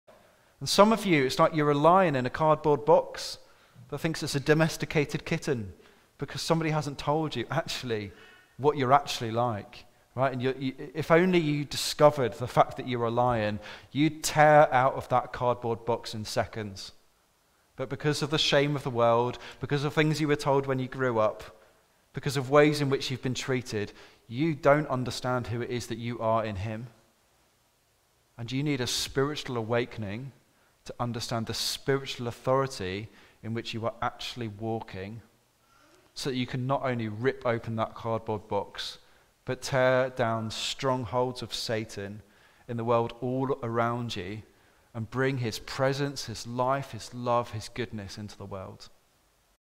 Hope Live | Exodus 29 | Some Priests Don’t Wear Robes
What an amazing message from Jamie 🦁📦

And finally, if you would like 1-TO-1 Prayer, message this page and we will connect you to someone from the prayer team…